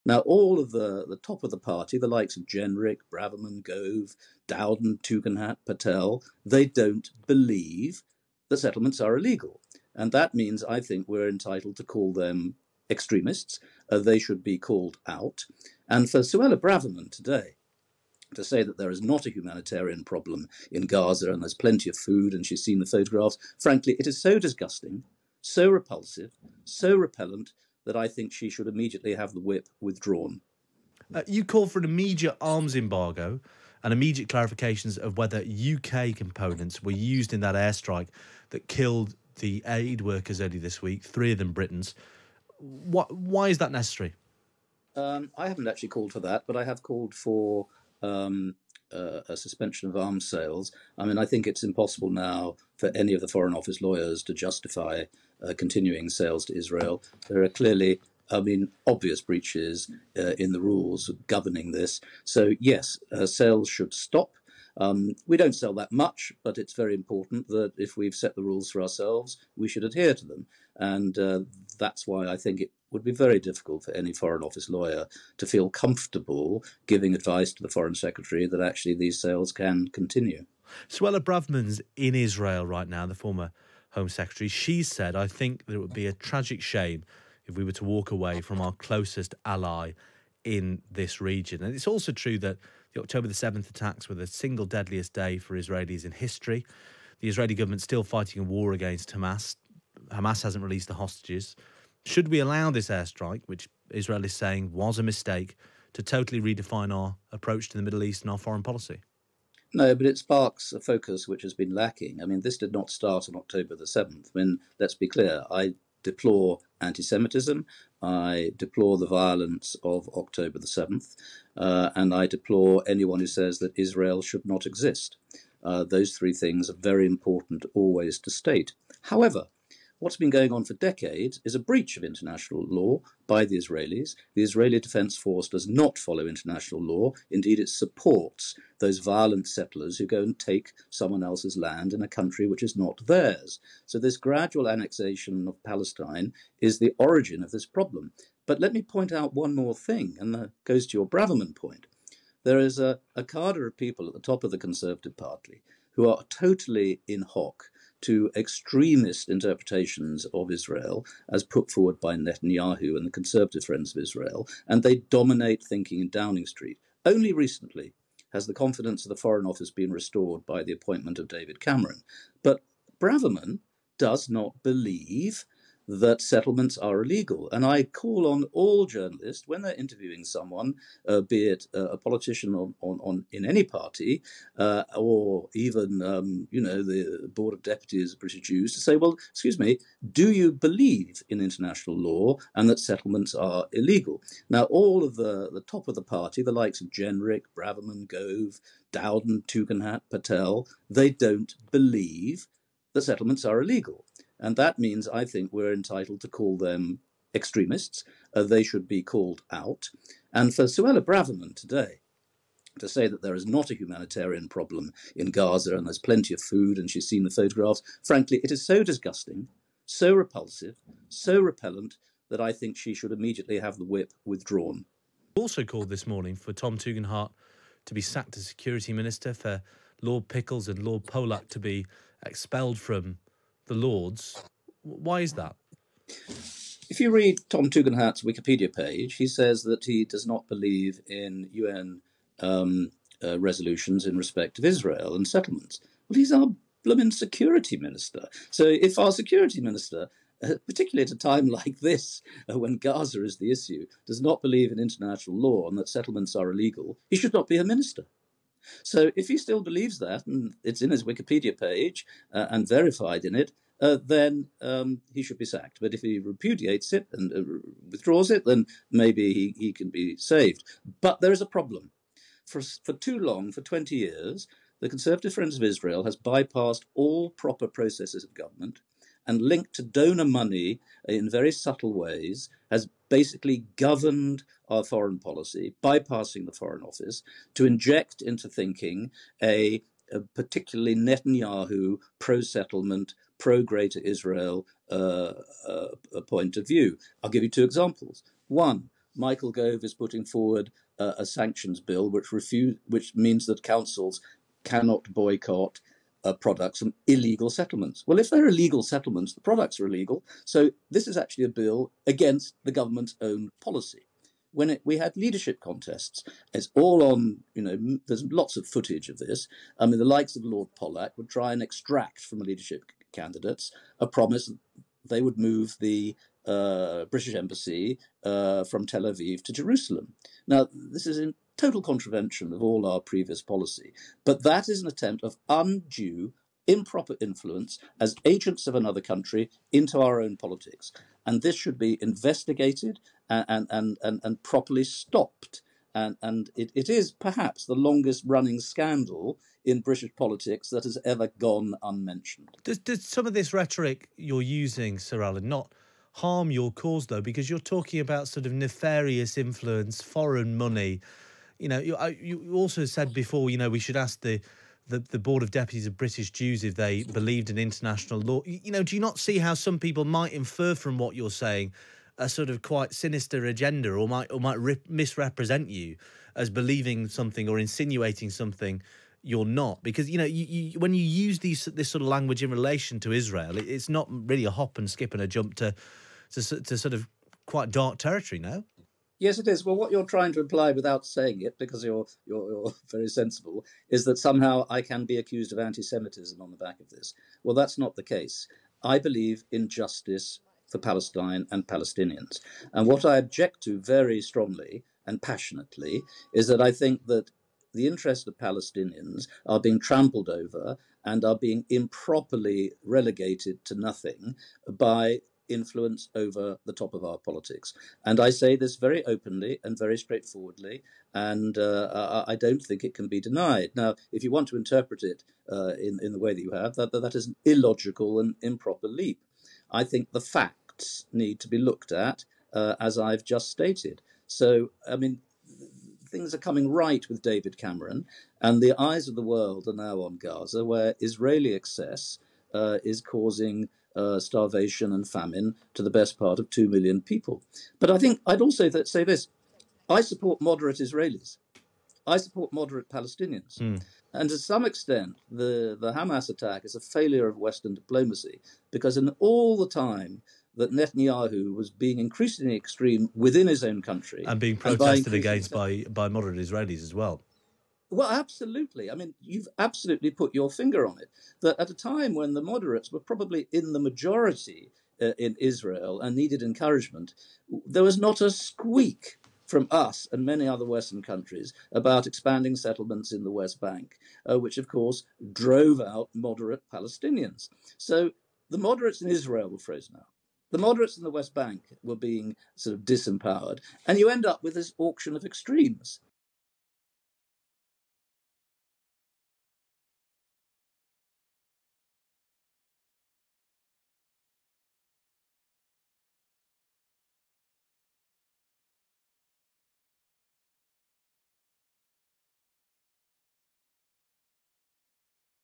Suella Braverman should 'immediately' have the whip removed | Sir Alan Duncan
“She's seen the photographs. Frankly, it is so disgusting, so repulsive, so repellent that I think she should immediately have the whip withdrawn.”

Sir Alan Duncan calls for Suella Braverman to have the whip removed for her "extreme" views on the humanitarian situation in Gaza.